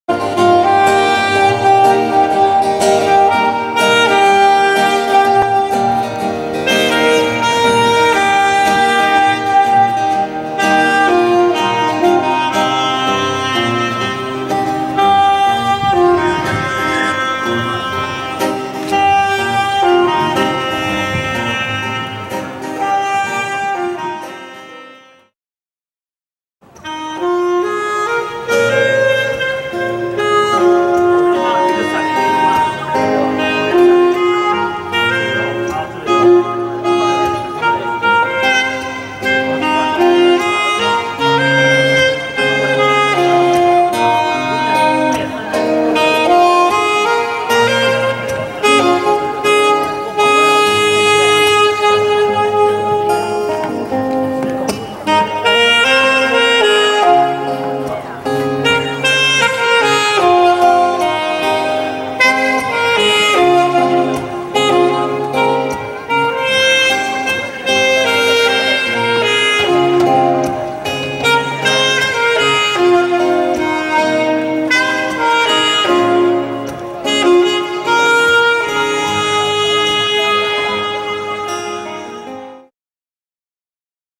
悸動石門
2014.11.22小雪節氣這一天
水庫風和日麗
在依山閣值班的我
耳邊不時傳來薩克斯風樂章
風中溢滿咖啡香
不覺中
心底裡隨著音樂的起伏
隨著香味的襲入
悸動~~~